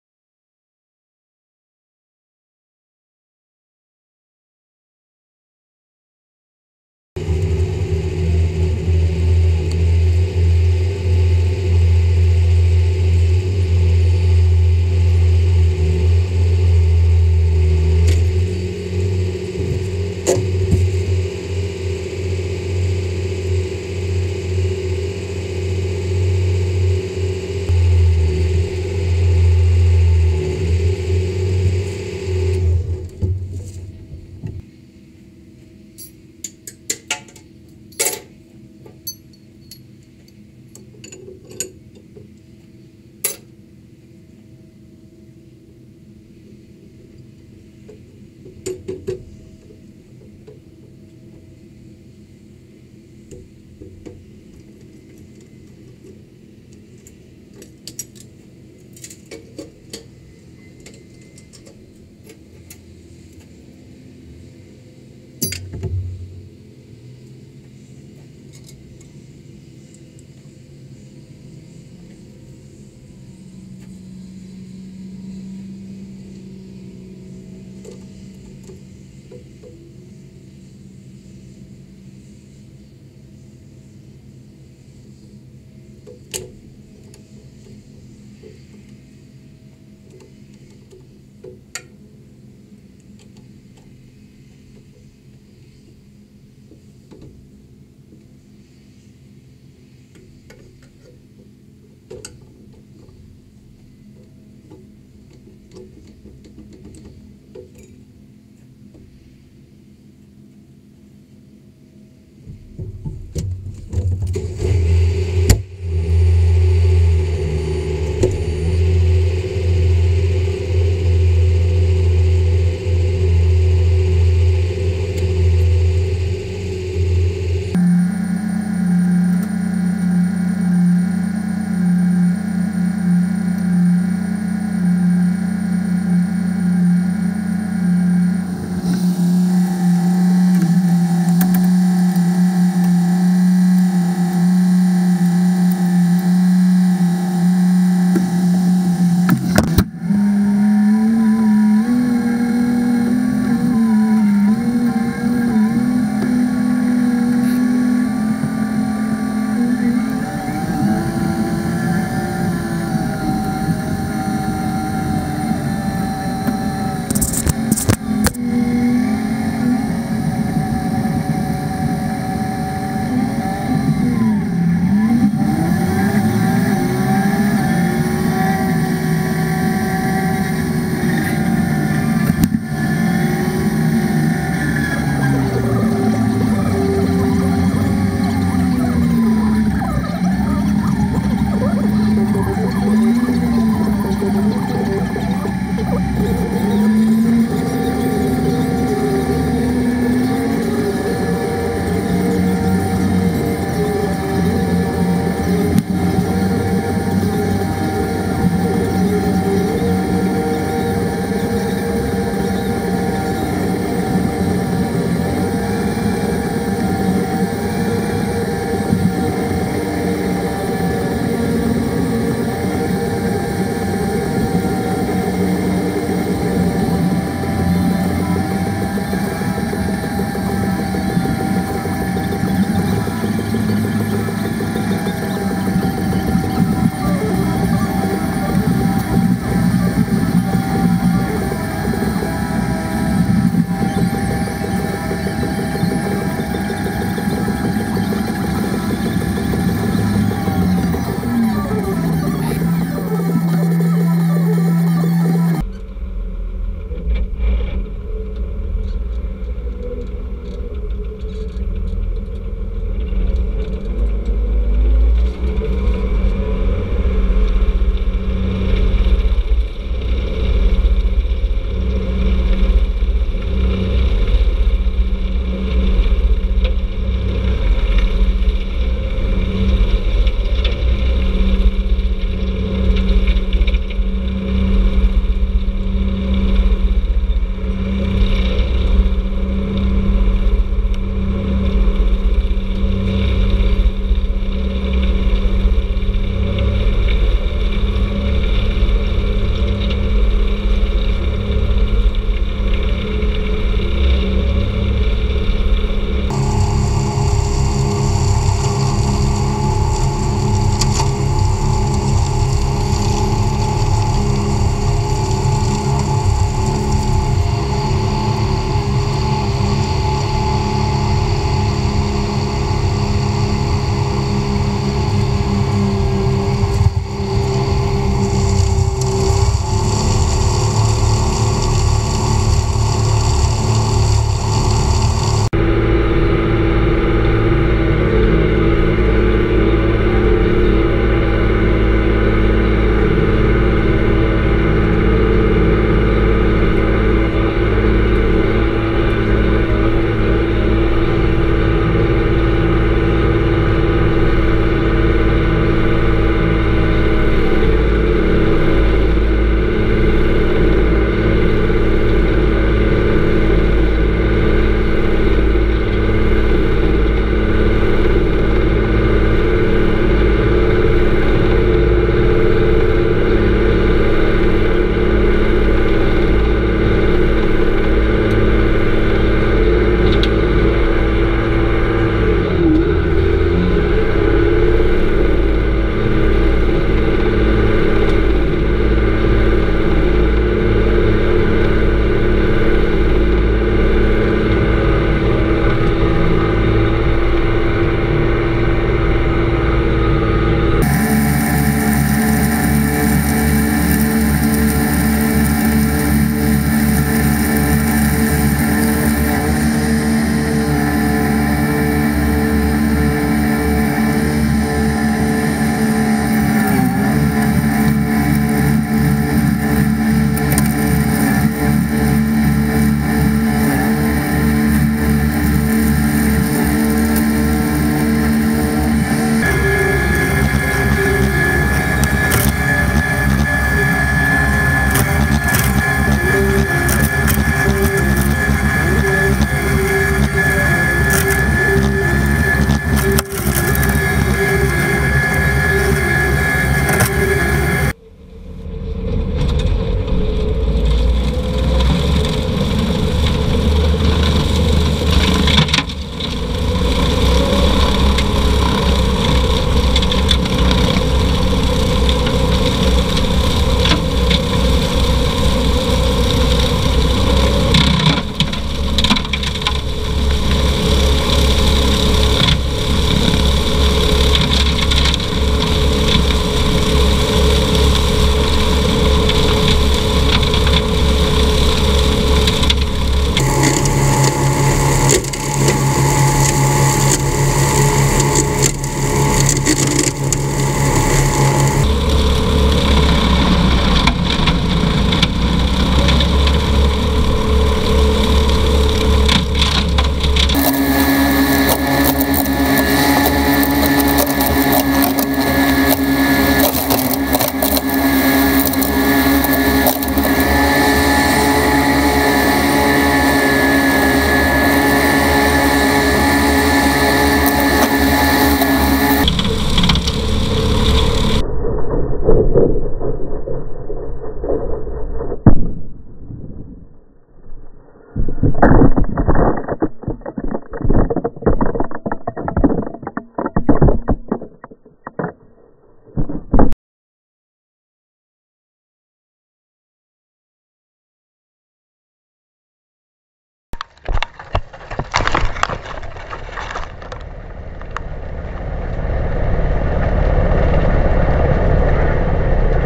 Case MXM 140 and Woods Batwing Mower (stay till the end)
Case 140 mowing down the pumpkin path from 2014 and a little incident.